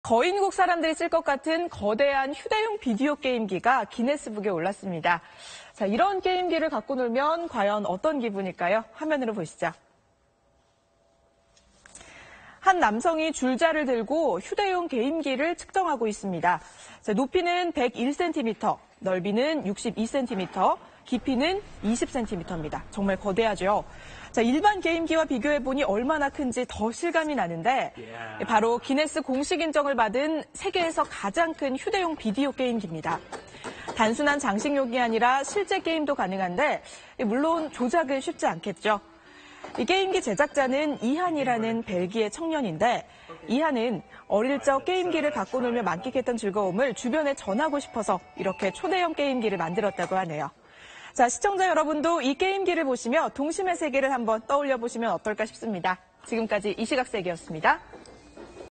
기네스에 등재된 '어마어마한 게임기' / SBS
거인국 사람들이 쓸 것 같은 거대한 휴대용 비디오 게임기가 기네스북에 올랐습니다. 이런 게임기를 갖고 놀면 과연 어떤 기분일까요? 화면으로 보시죠.

한 남성이 줄자를 들고 휴대용 게임기를 측정합니다. 높이 101cm, 넓이 62cm, 깊이는 20cm. 정말 거대하죠? 일반 게임기와 비교해 보니 얼마나 큰지 더 실감이 나는데 바로 기네스 공식인정을 받은 세계에서 가장 큰 휴대용 비디오 게임기입니다.